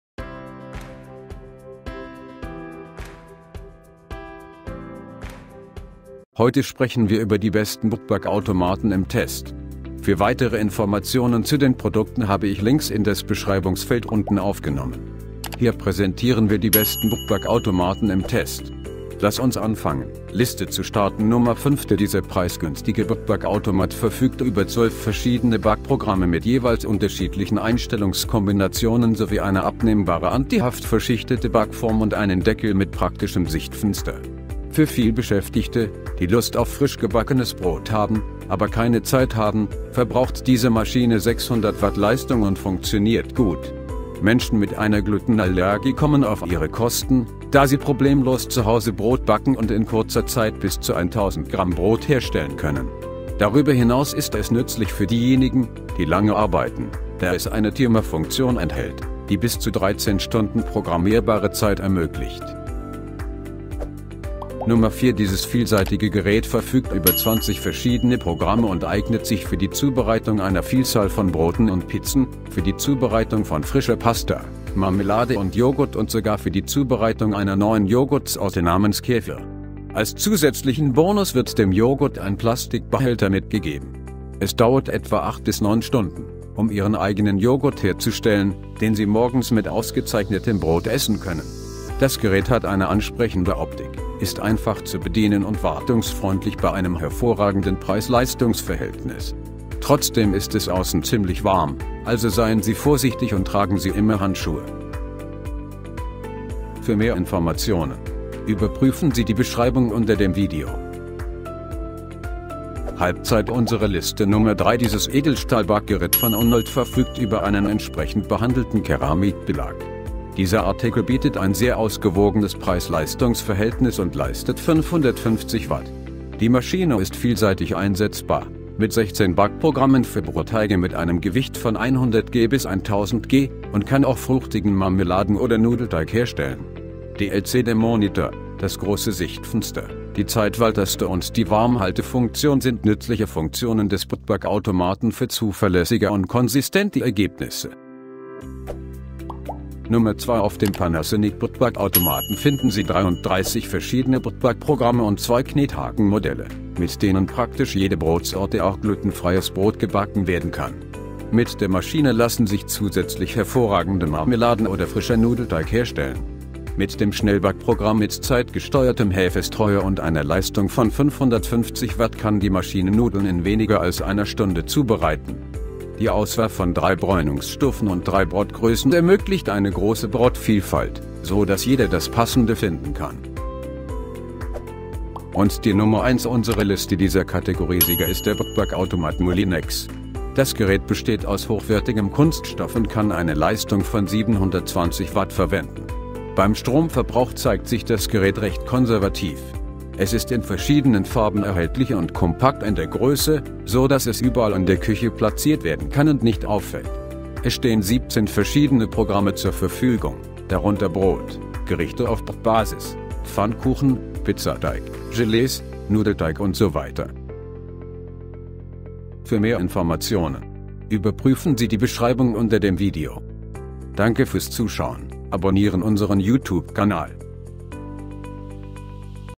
✅ Brotbackautomaten Test 2022 - Die besten Brotbackautomaten Bewertungen
Ich habe diese Liste basierend auf meiner persönlichen Forschung und Meinung und ich habe versucht, sie basierend auf Preis, Qualität, Haltbarkeit und mehr aufzulisten.

Hier ist die Liste von Title.

✅ 1. Moulinex Machine à OW210130 Pain
✅ 2. Panasonic Brotbackautomat
✅ 3. UNOLD Brotbackautomat Backmeister Edel
✅ 4. Moulinex Brot/Flower Delight Brotbackautomat
✅ 5. Clatronic BBA 3505 Brotbackautomat